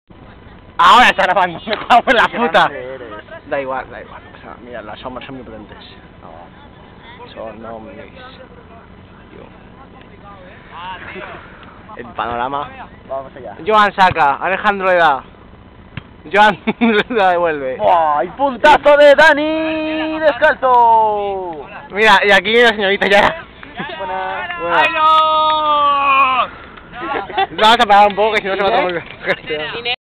Voley en la malvarrosa 2ª parte
Otro intento del ser humano por meter un maldito punto, mientras ocurre la esperada llegada de Yara, jaja!!